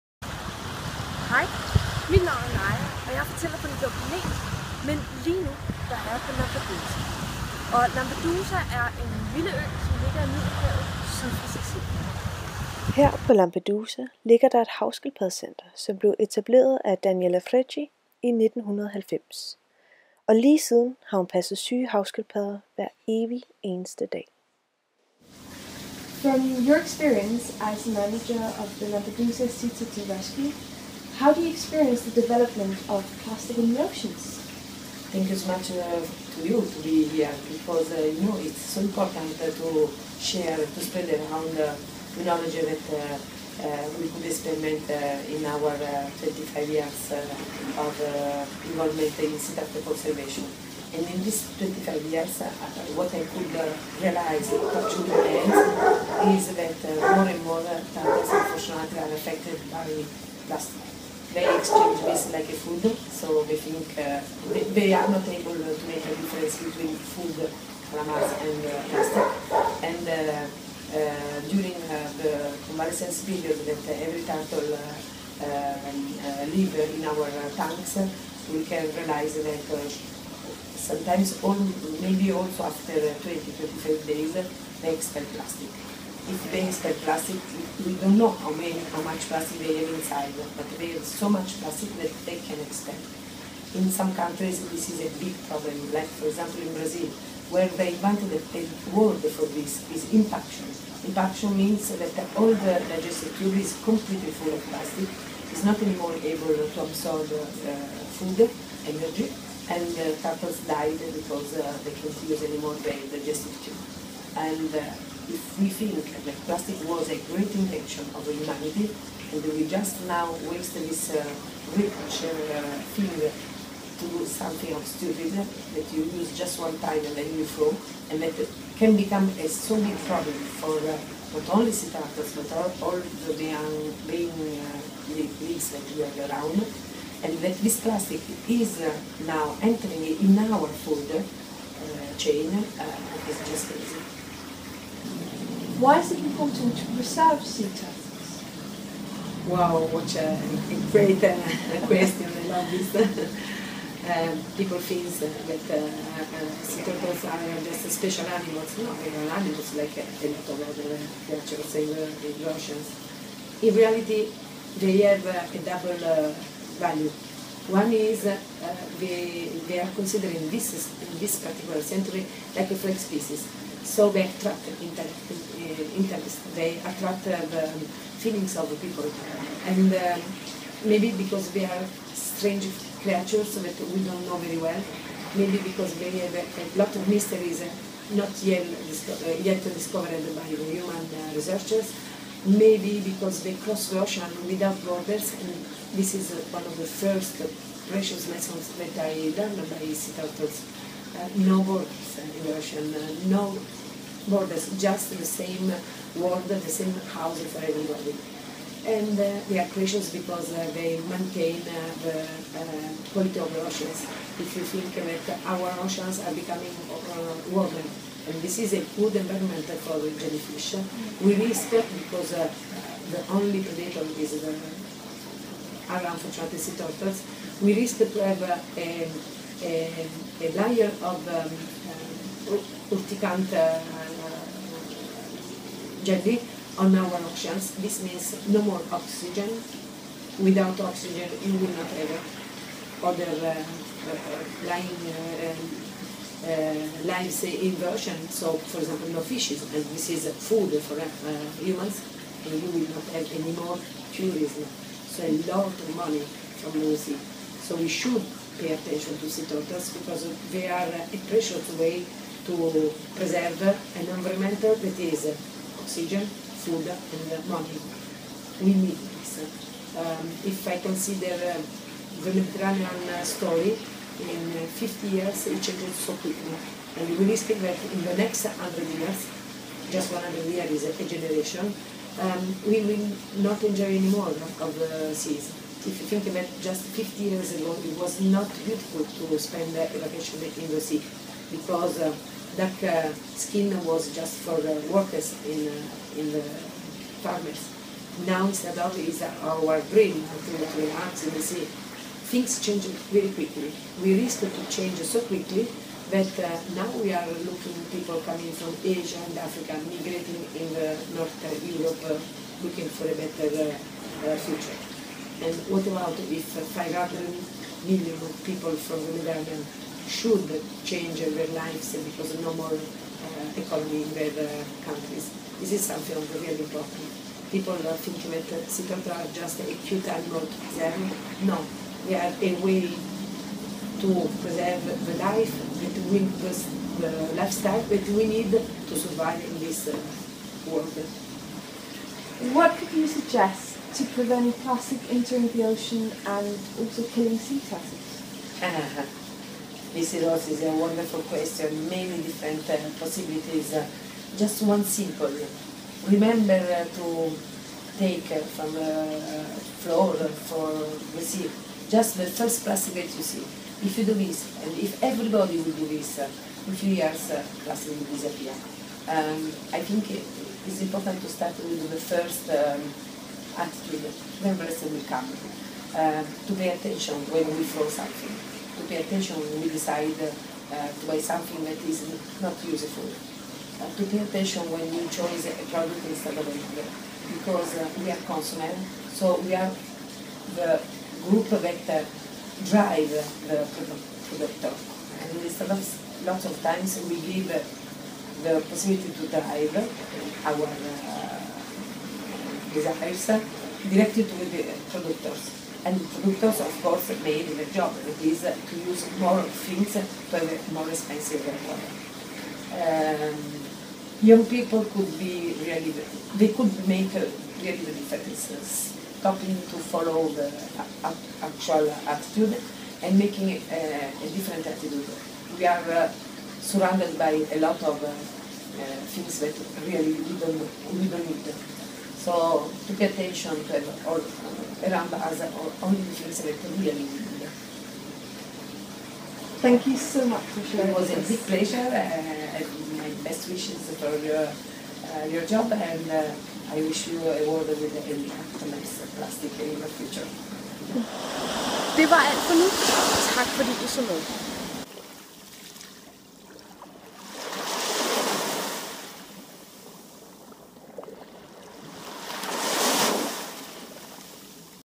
Ildsjæle redder havskildpadder
Den Blå Planets fortæller Naja Bertolt Jensen besøger Lampedusa Turtle Rescue, der redder havskildpadder.